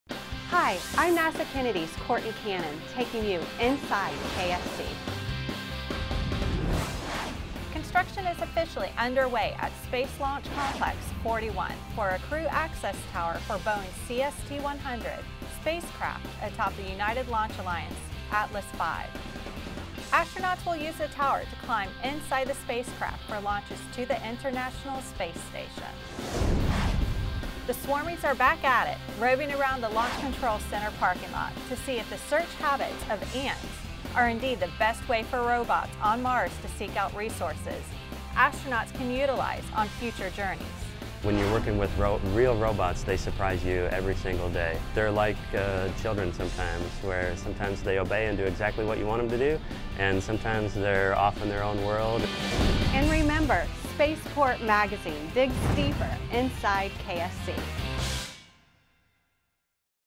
Inside KSC! Feb. 27, 2015
Boeing and ULA broke ground Feb. 20 on a new crew access tower for Commercial Crew in this week's Inside KSC! Also, take a look at the progress of the "Swarmies" as they test search techniques that could be used in future exploration.
Written by Steven Siceloff
Produced by Aaron Peshek
Edited by Chris Chamberland